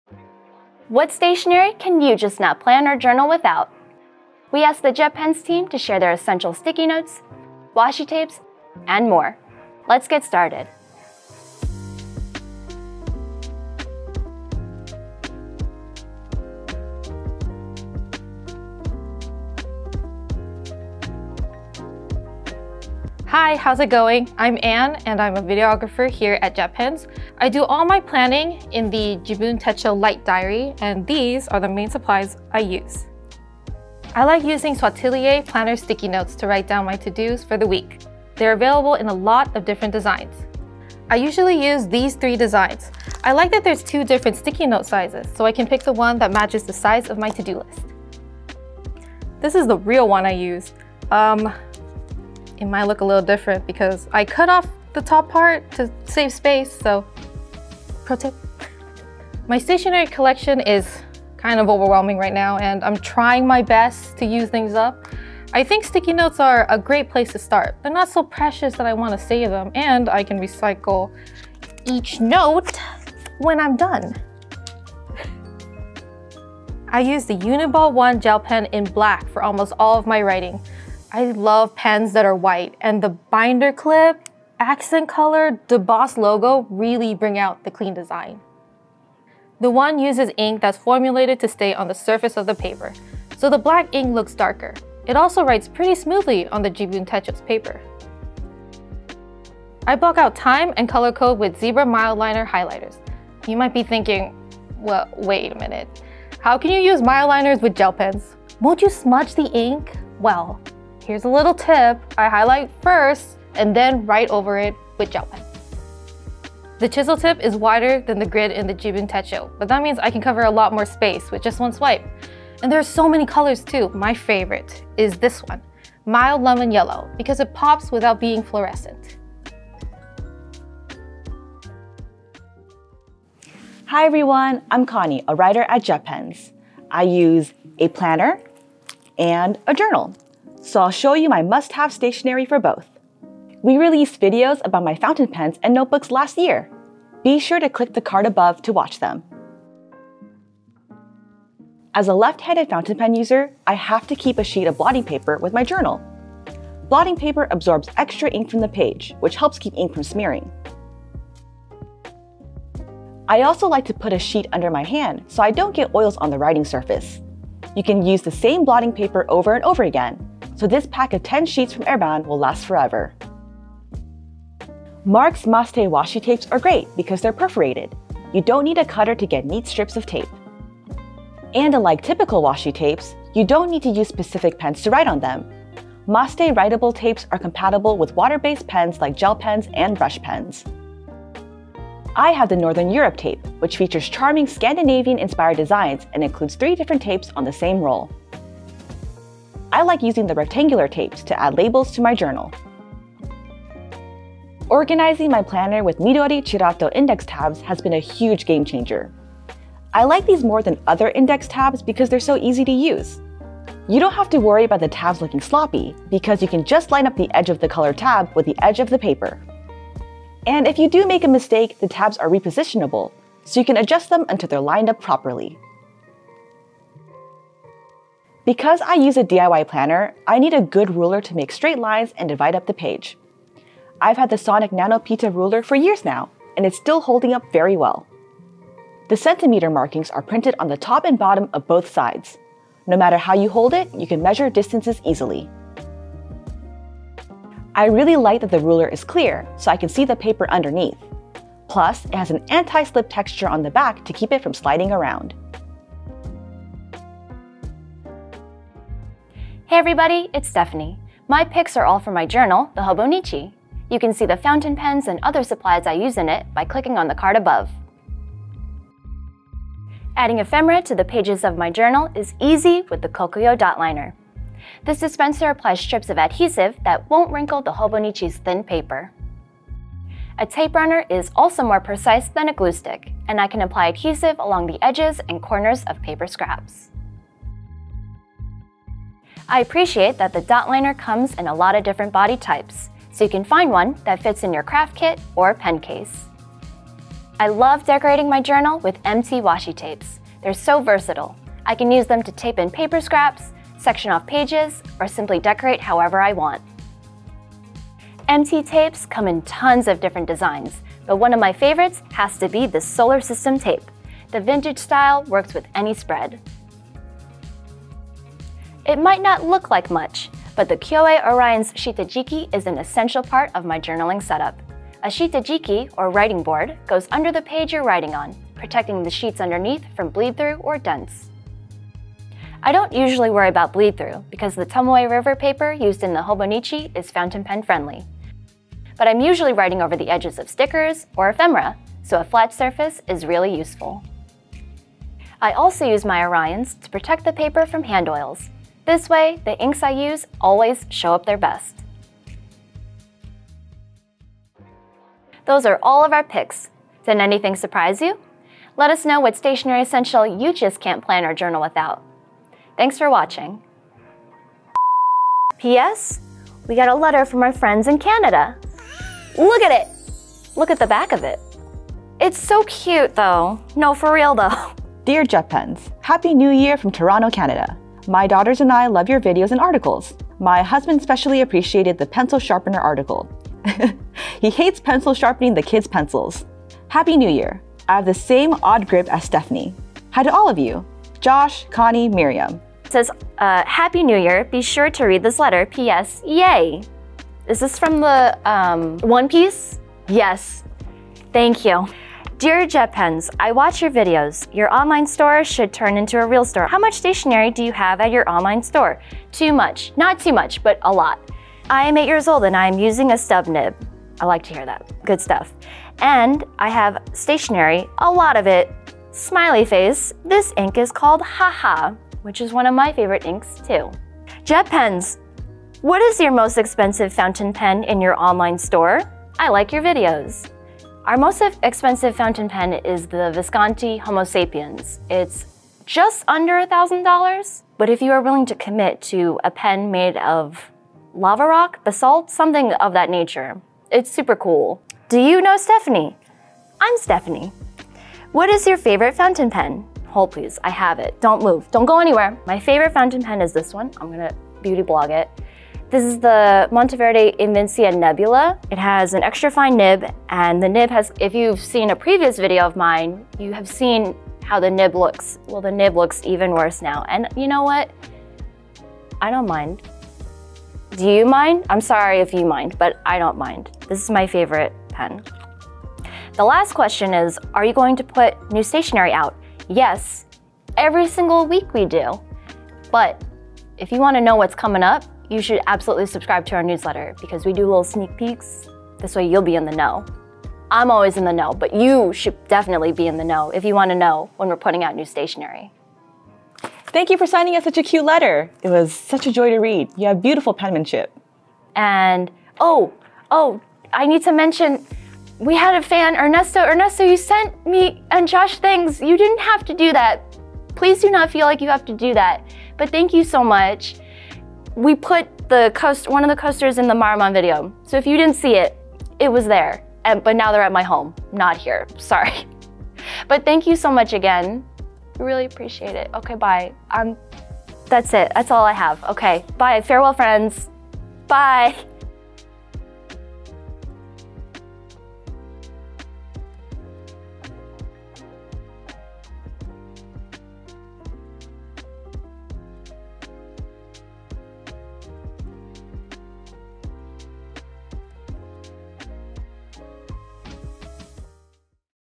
BUDGET Journal Supplies We CAN'T Live Without!
We asked the JetPens team: what stationery can you just not plan or journal without? Ann, Connie, and Stephanie shared their essential writing instruments, sticky notes, and more.

0:00 Intro
0:22 Ann's Picks
2:13 Connie's Picks
4:19 Stephanie's Picks
5:55 Outro
6:05 Easter Egg